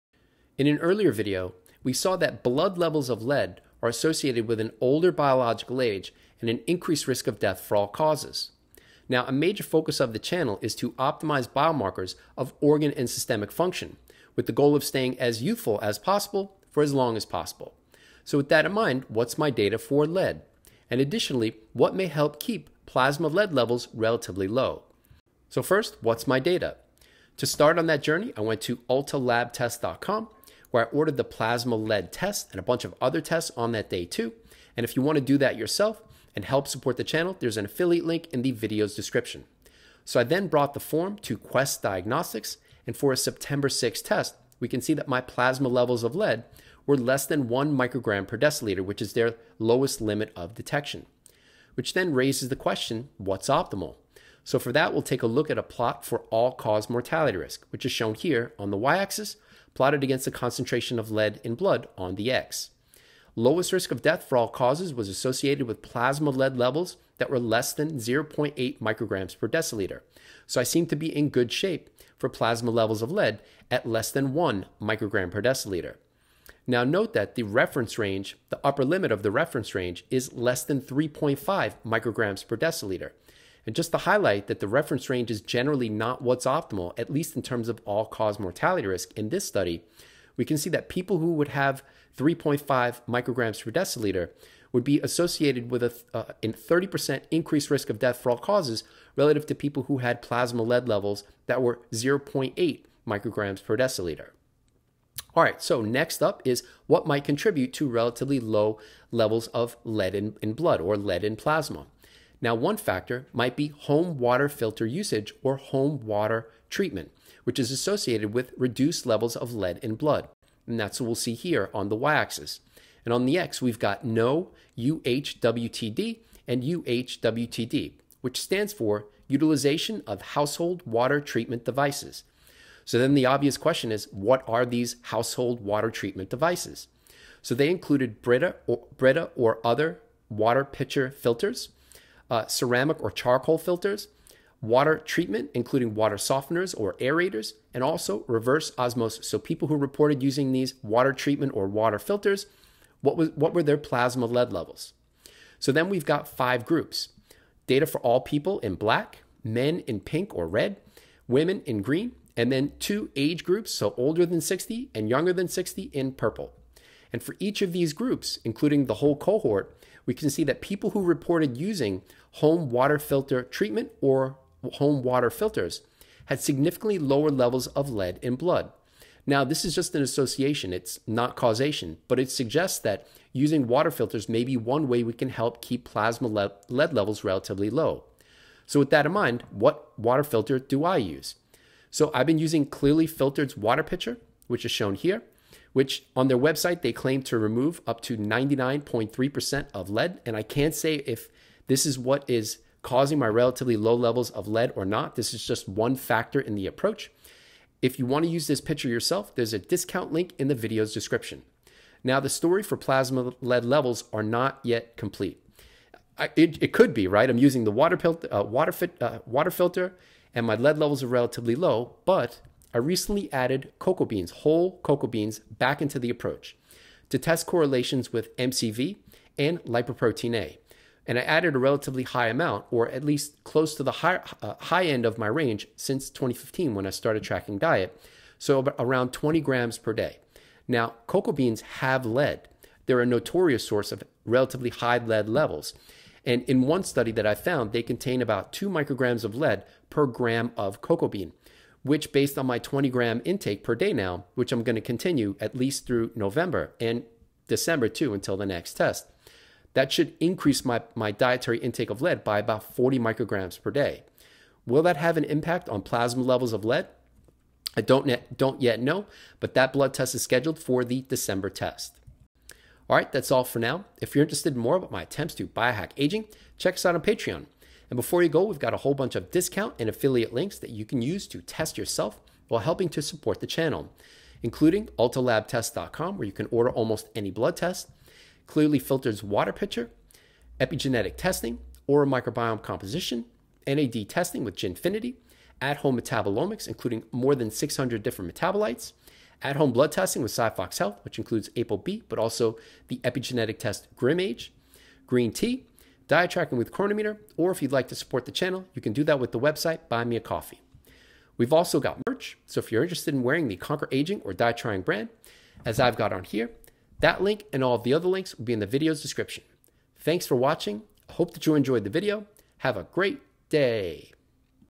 Home Water Filter Use Is Associated With Less Lead In Blood
Papers referenced in the video:
Low-level lead exposure and mortality in US adults: a population-based cohort study

Associations of the utilization of household water treatment devices with mortality